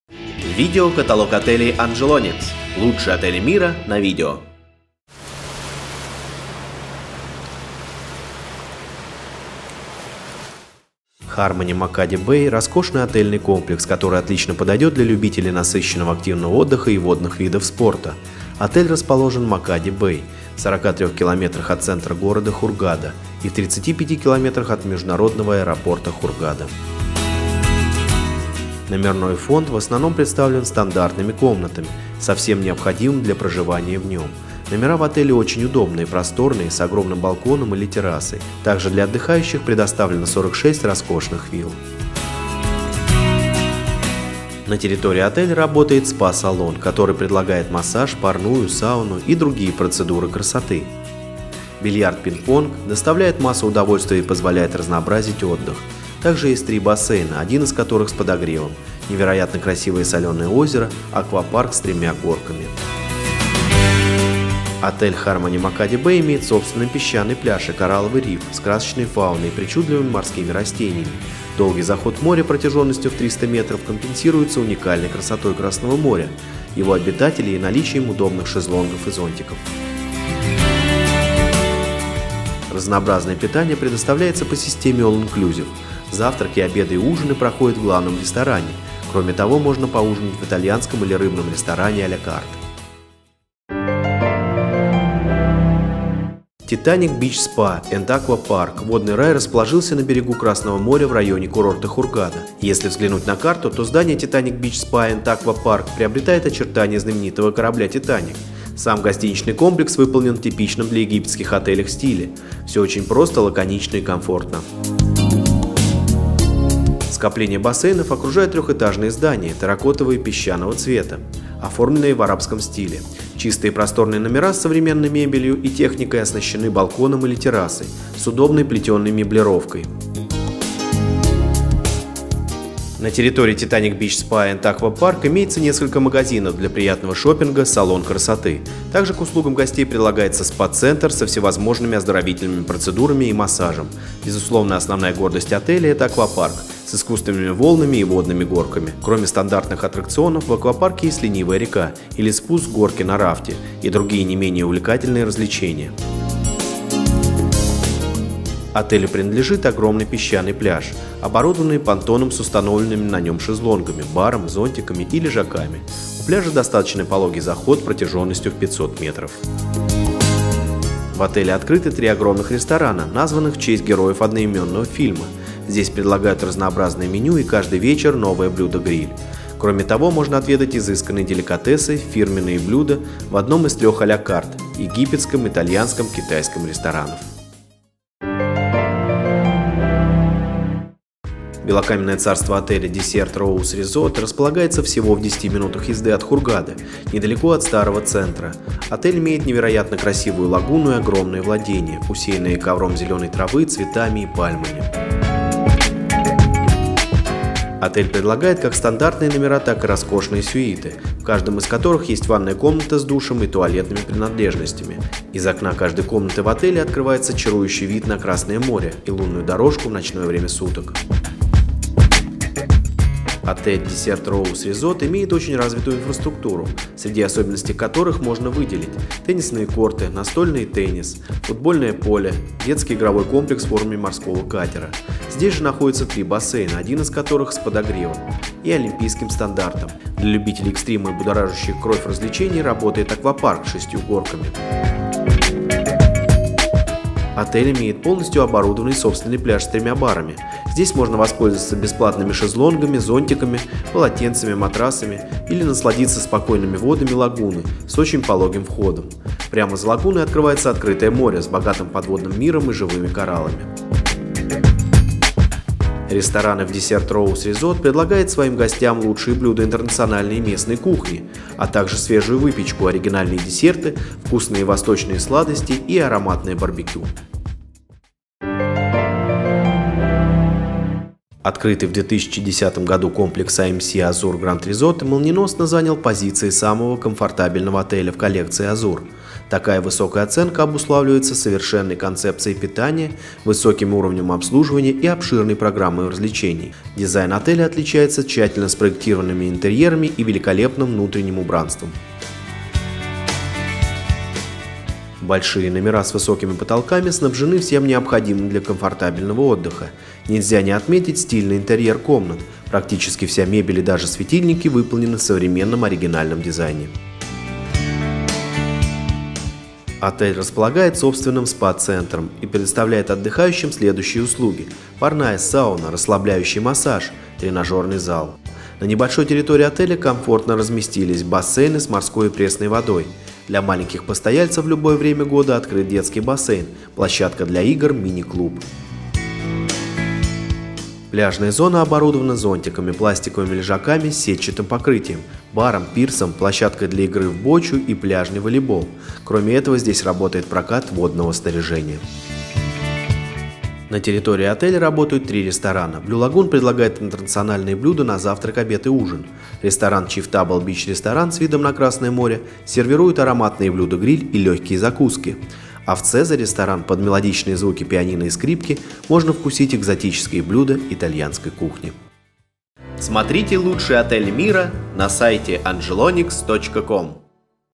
Отели Хургады 5*
Harmony Makadi Bay роскошный отельный комплекс, который отлично подойдет для любителей насыщенного активного отдыха и водных видов спорта. Отель расположен на Makadi Bay, в 43 км от центра города Hurghada и в 35 км от международного аэропорта Hurghada. Harmony Makadi Bay роскошный отельный комплекс, который отлично подойдет для любителей.

Titanic Beach Spa & Aqua Park -- «водный рай», расположился на берегу Красного моря в районе курорта Хургада. Если взглянуть на карту, то здания Titanic Beach Spa & Aqua Park приобретают очертания знаменитого корабля «Титаник». Сам гостиничный комплекс выполнен в типичном для египетских отелей стиле: все очень просто, лаконично и комфортно...

Белокаменное царство отеля Desert Rose Resort располагается всего в 10 минутах езды от Хургады, недалеко от Старого центра. Отель имеет невероятно красивую лагуну и огромные владения, усеянные ковром зеленой травы, цветами и пальмами...

Открытый в 2010 году комплекс AMC Azur Grand Resort молниеносно занял позицию самого комфортабельного отеля в коллекции Azur. Такая высокая оценка обуславливается совершенной концепцией питания, высоким уровнем обслуживания и обширной программой развлечений. Дизайн отеля отличается тщательно спроектированными интерьерами и великолепным внутренним убранством...